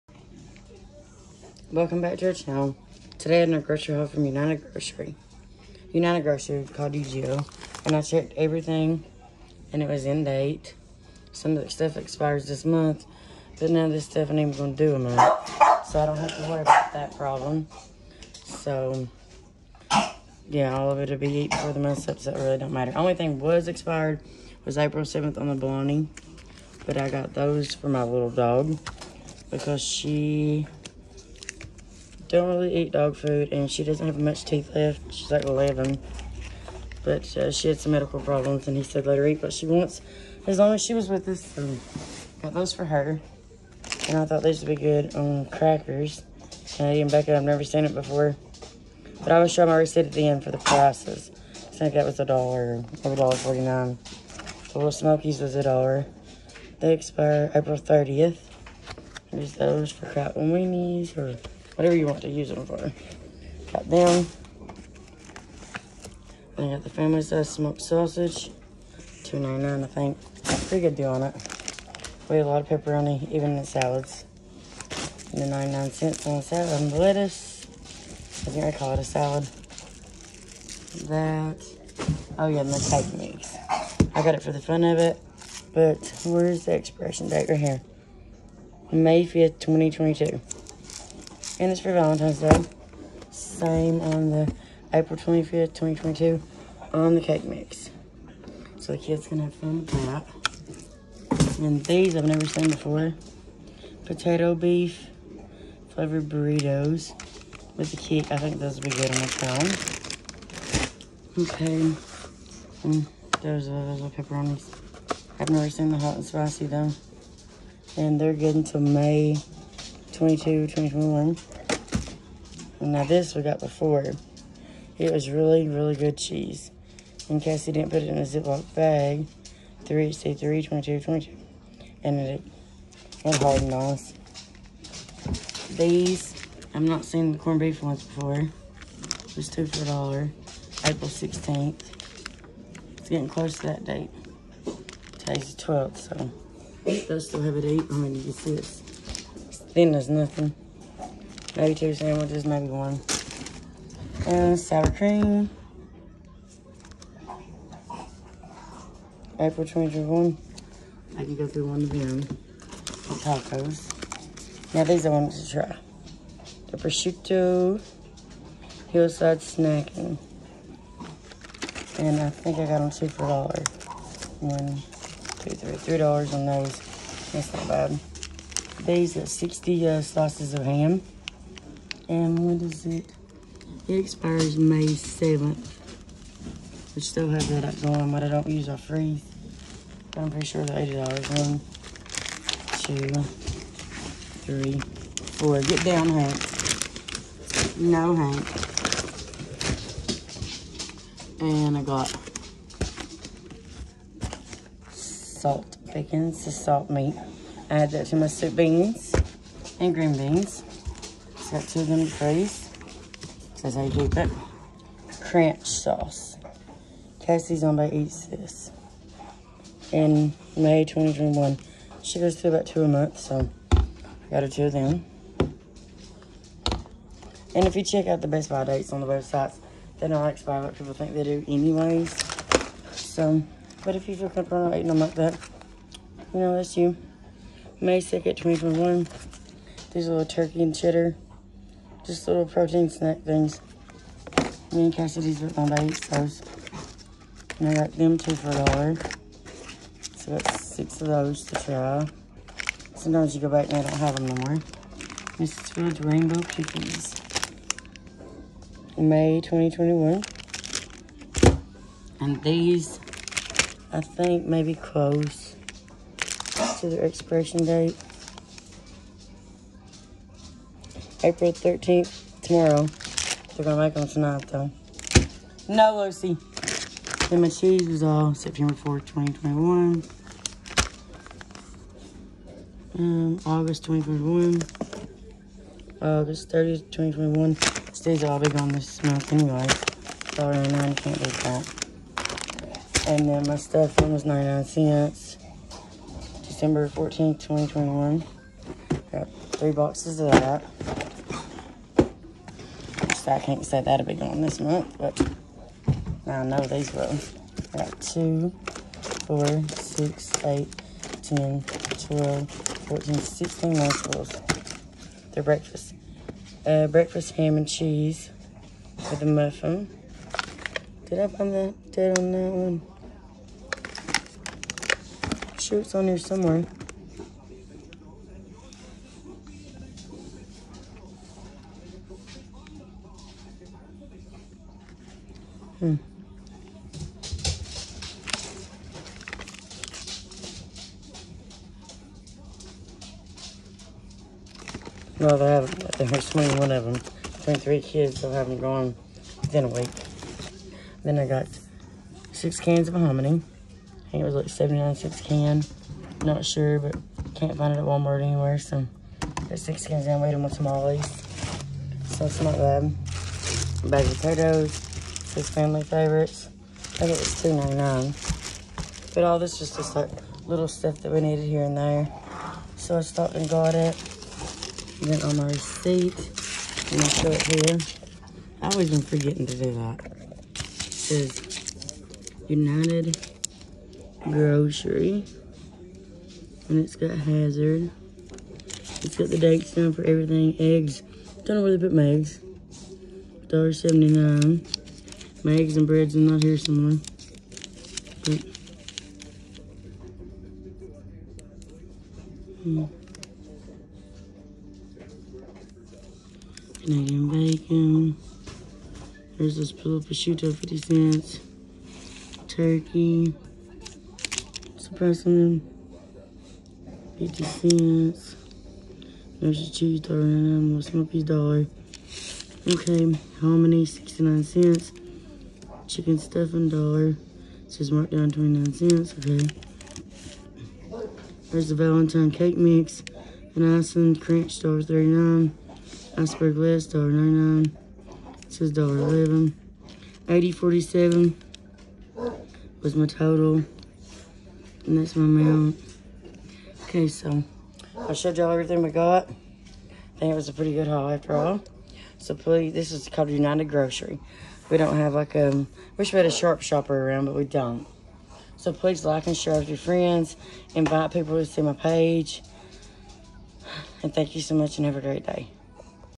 United Grocery haul UGO all this for $80.47
Pretty good haul for the price and some really good deals. Trying to grow this channel. My first videos please. Tell me of the stuff you would like to see.